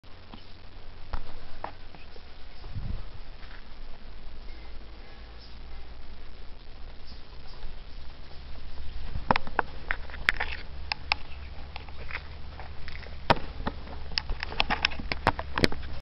DSCF6285 UKRAINA Pologi kolo Ochtyrki Dom,,zony,,
We wsi Pologi Rejon Ochtyrski oblast sumska na Ukrainie!Odnalazlem,,wiezienie,, mej corki i Azyl ,,zony,, z pasierbem-gdzie za moje pieniadze i po  zmarlej matce-ukradzione z konta kupila na pasierba dom!(Bo zazdroscil mej coreczce majatku!)